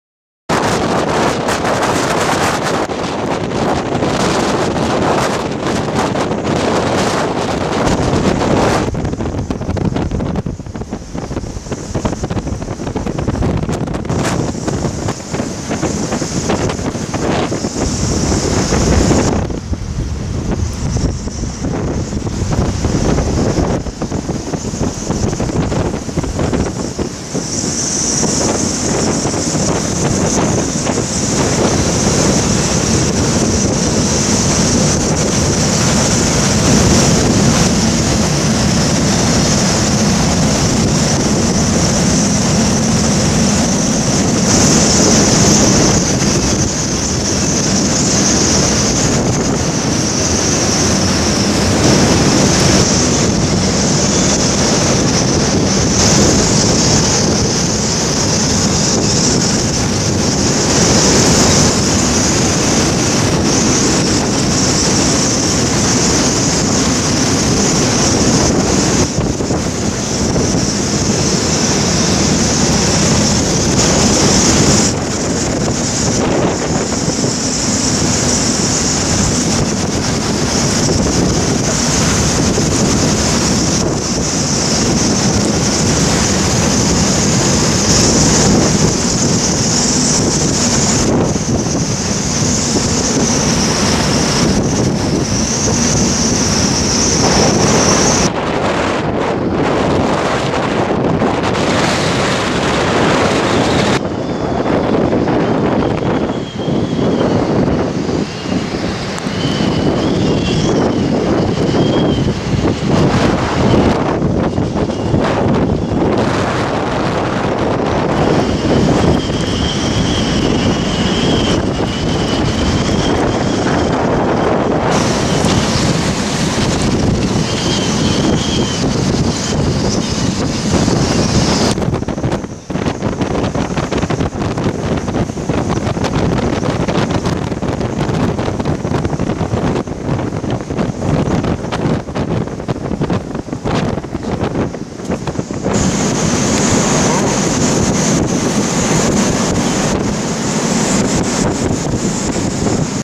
16 DEC 2011 BELGIAN COAST WESTENDE HEVIGE STORM STEEKT KOP OP PART 2
WESTENDE 12.45 H - HEAVY STORM PART 2 (Sony Handycam)